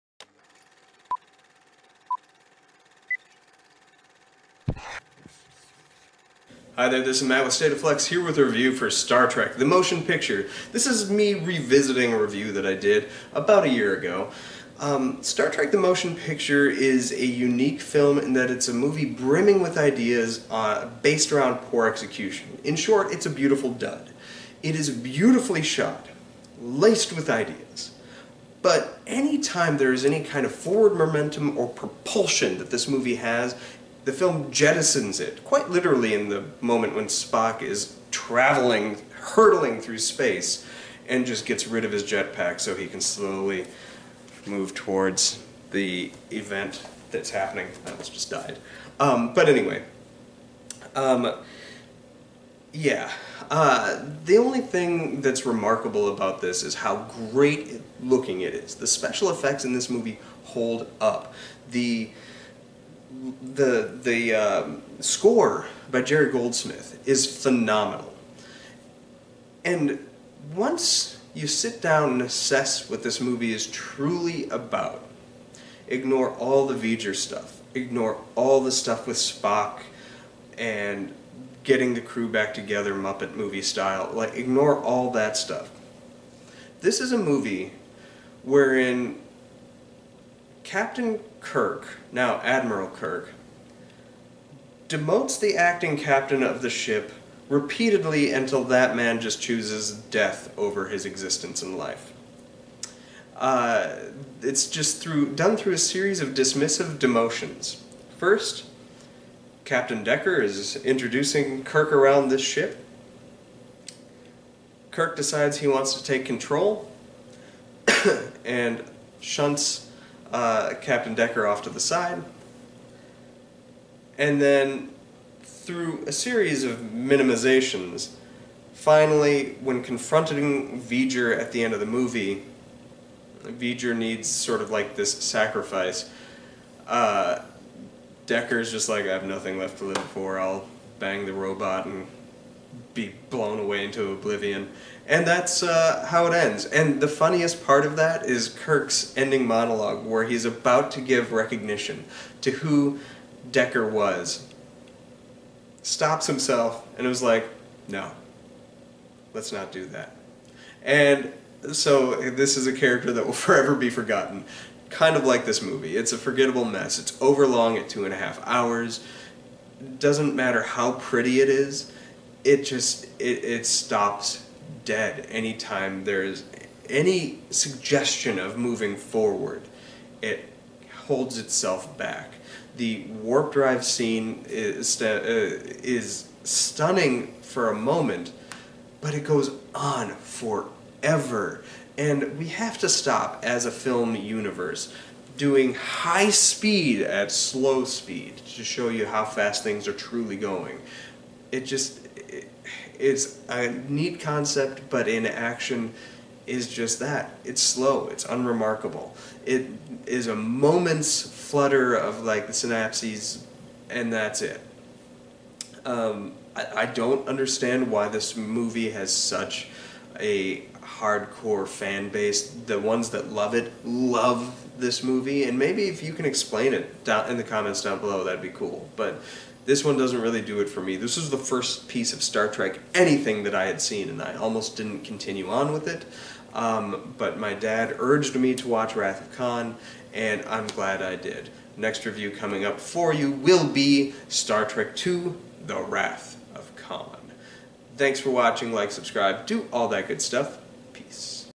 Star Trek the Motion Picture Revisit
Taking a briefer look at the first Star Trek film.